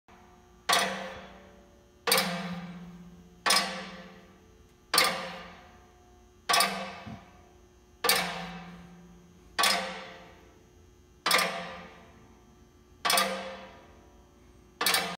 Anatol Tornado Riding My Press!!! Screen Printing Fun!!!
At Anatol we take testing to a whole new level!!! Some fun out takes from the soon to come video!!! ***do not try this I am a trained certified technician and this should never be attempted and am not responsible if you are too dumb to listen***